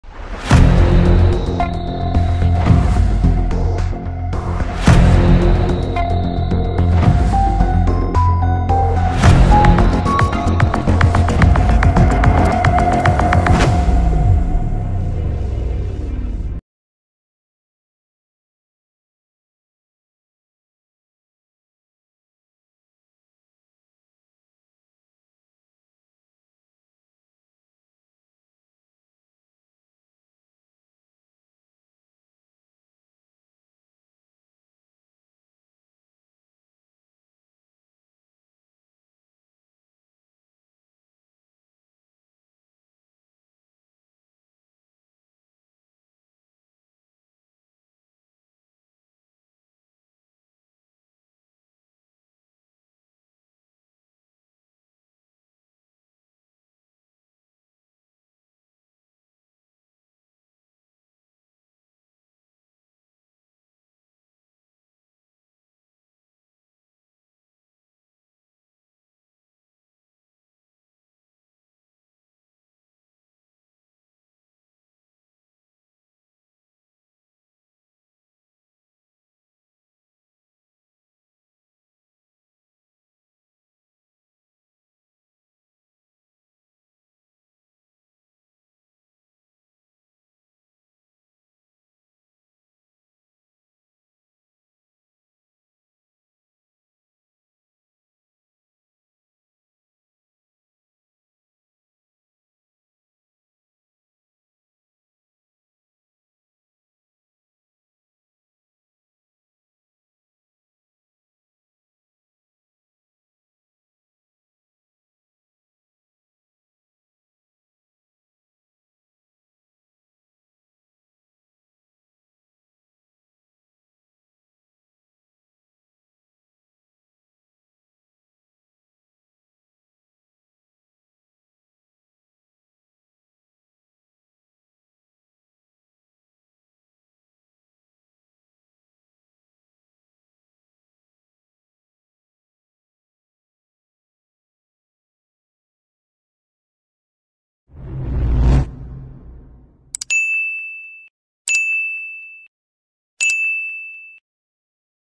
Ruined Rainbow Reality Season 3 Episode 12: A Promise Kept…//My AU
Light informs Proxima, Yamel, and Malik that they couldn’t find him. Meanwhile Nebula contacts someone, informing them that Sabre is with them.

Audio:
CapCut - All Music